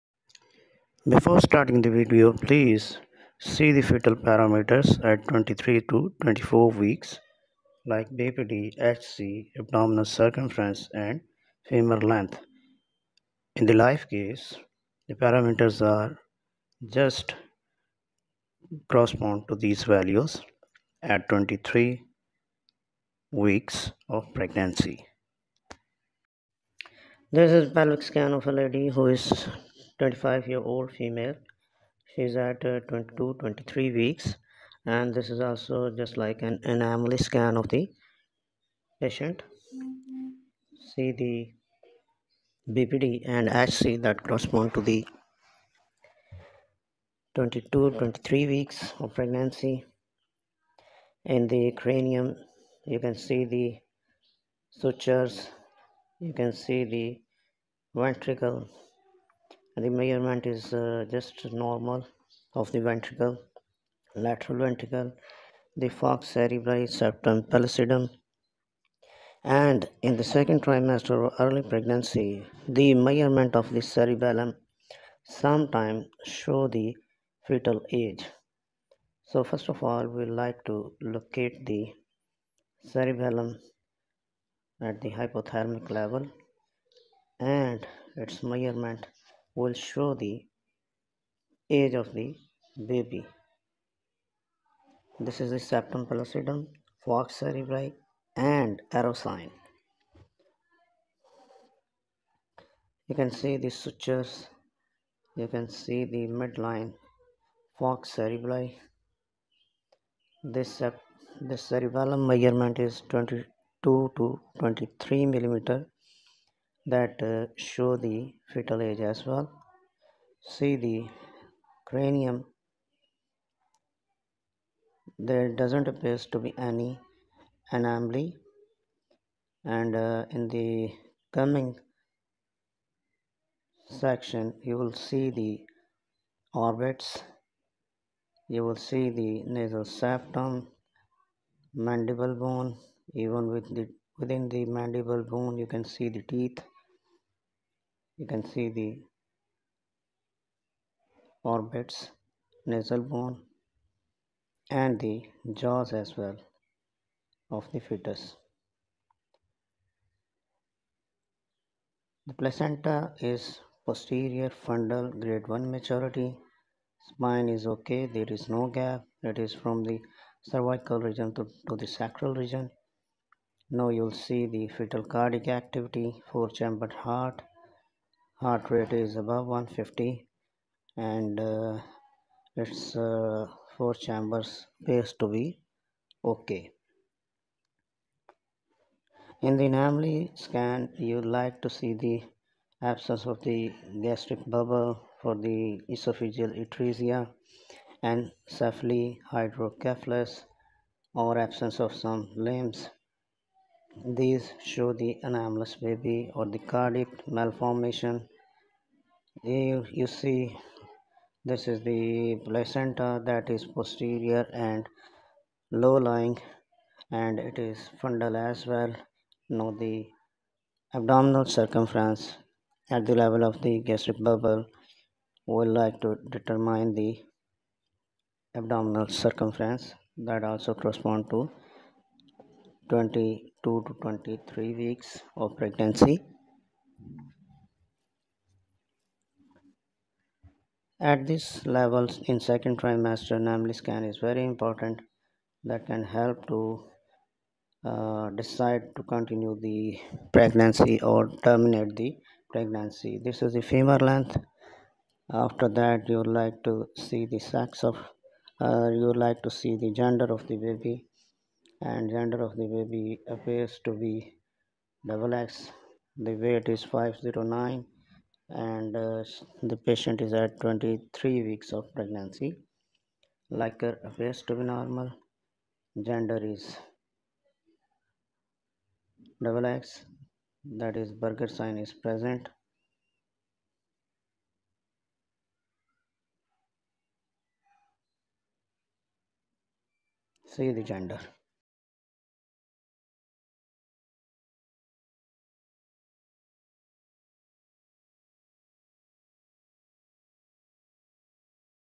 FETAL PARAMETERS AT 23 WEEKS 🔥❤l BPD l HC l FL l😇
Fetal parameters at 23 weeks I bpd I hc I fI I/Fetal parameters in hindi.
In this video I am teaching about the gender determination, fetal parameters, heart❤ rate, abdominal circumference, fetalAFI ETC.